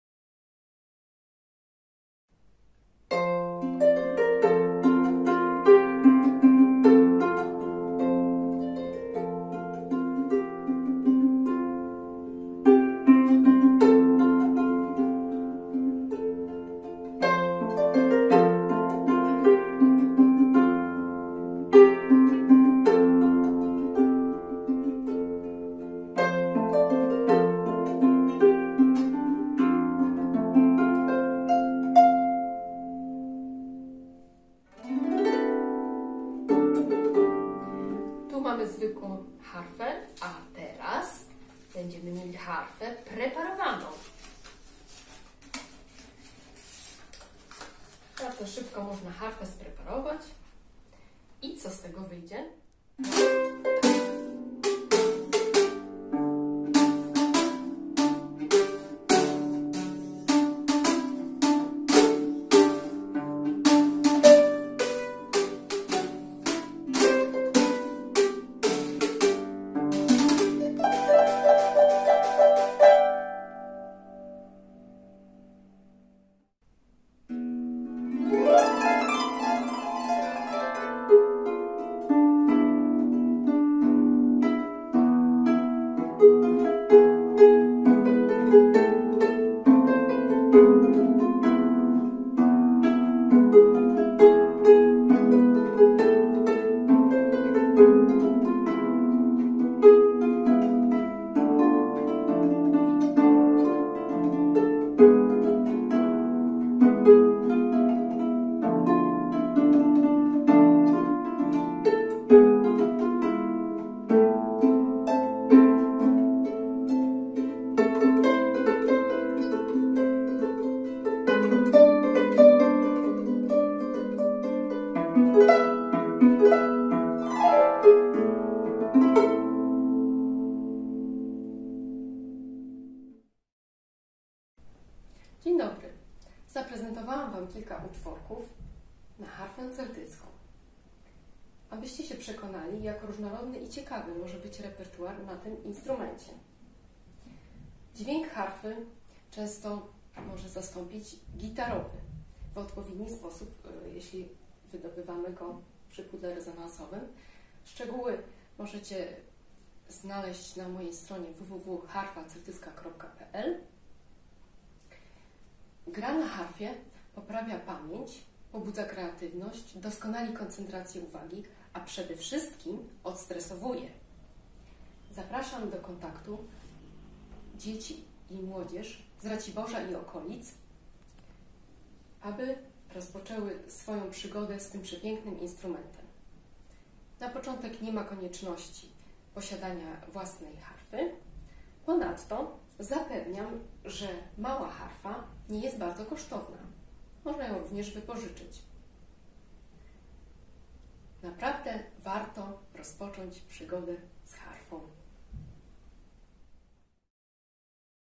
Zachęta do nauki gry na harfie celtyckiej
Nauka już od 6 go roku życia
Młodzież relaksuje się przy dźwiękach harfy
Osoby dorosłe odkrywają swoje możliwości